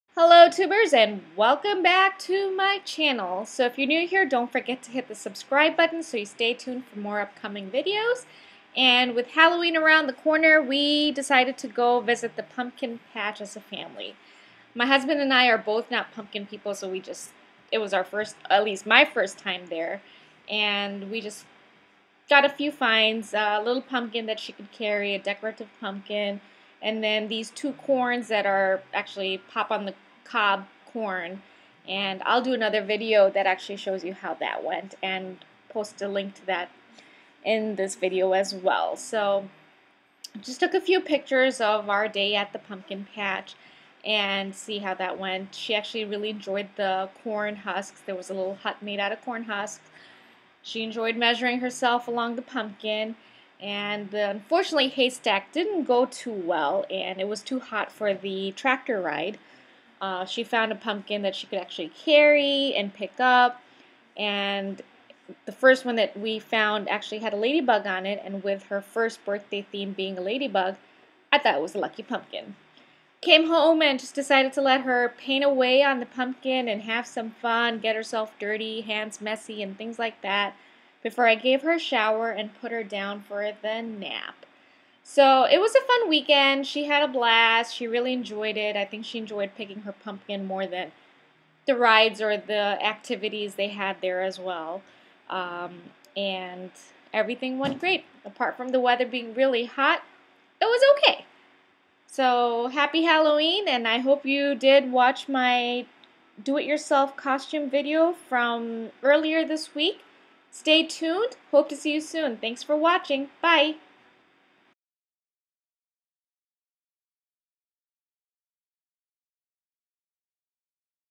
Trip to the Pumpkin Patch October 2016
My daughters and my first time at the pumpkin patch.  We had a lot of fun especially painting the pumpkins after coming home.  Despite the heat she was really excited to be there.